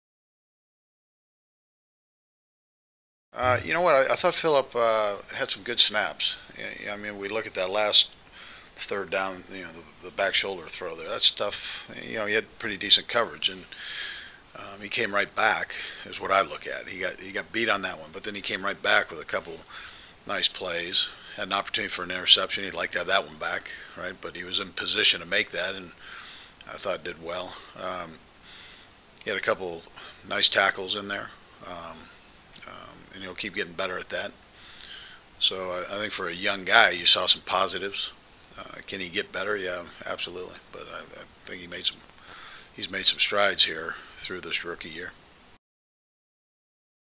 Chiefs Coach Andy Reid on cornerback Phillip Gaines
Chiefs Coach Andy Reid talks about cornerback Phillip Gaines and thinks he has made some good strides in his rookie season with The Kansas City Chiefs.